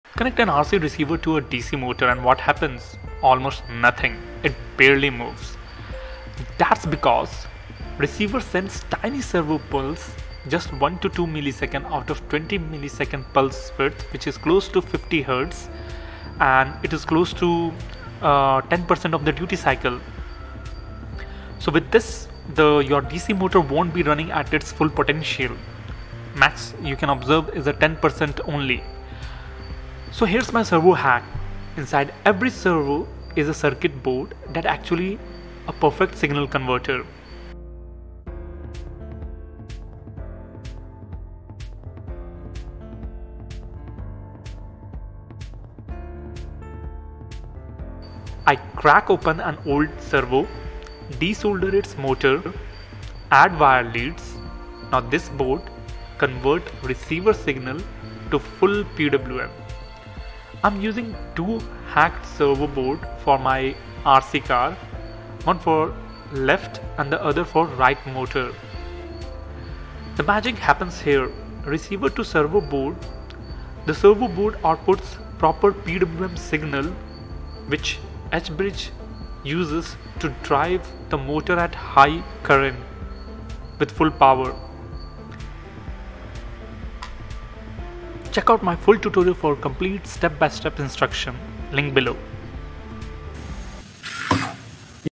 Control DC Motors With ANY RC Controller
🔧 The Ultimate Servo Hack - Convert RC Signals to PWM for DC Motors!
Turn any old servo into a signal converter and save money on expensive ESCs! In this quick tutorial, I show you how to hack servo circuit boards to get full variable speed control of DC motors using standard RC receivers.
⚡ What You'll Learn:

Why RC receivers can't directly control DC motors
How to extract and repurpose servo circuit boards
Converting 1-2ms servo pulses to 0-100% PWM signals
Proper wiring to H-bridge motor drivers
Calibrating neutral position with the potentiometer

💡 This hack is perfect for:

DIY RC cars and boats
Arduino robotics projects
Any project needing variable DC motor control
Saving money on Electronic Speed Controllers (ESCs)

🛠️ Parts Used:

Old servo motors (any brand works)
H-bridge motor driver module
RC receiver (FS-i6 or similar)
DC motors
Basic soldering supplies

Drop a comment if you try this hack! What will you build?